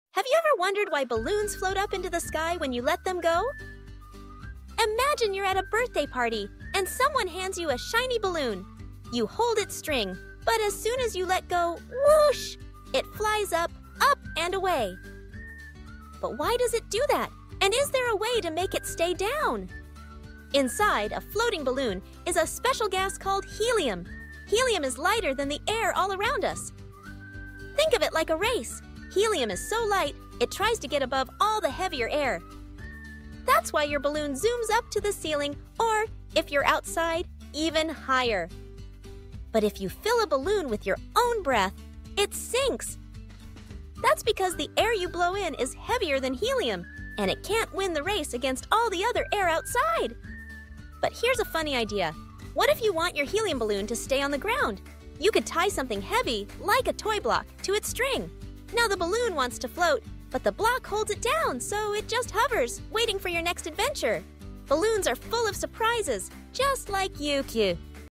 Why Do Balloons Float Up? | Fun Science Experiment for Kids | Helium, Buoyancy, Air Tricks
Have you ever wondered why balloons float up in the air? 🎈 In this fun experiment, @minilandboomers explains the science behind floating balloons and shows kids how they can make them stay down!

Why do balloons float up? And is it possible to make them stay down? 🎈 In this exciting @minilandboomers  video, we explore the science behind helium, air, and floating objects in a way kids can understand.

Balloons float because helium is lighter than air, and this simple concept introduces kids to the principles of buoyancy, density, and air pressure. But can you control where they go? With fun experiments, we’ll show how kids can make balloons float, hover, or stay down safely at home!

In this video, you’ll learn:

Why helium-filled balloons float and air-filled balloons don’t

How density and weight affect floating

Cool experiments to make balloons stay down

The fun science behind buoyancy and gases

How to observe and predict balloon behavior like a young scientist

Tips to make science interactive, hands-on, and magical

Perfect for kids who love experiments, fun learning, and magical science tricks!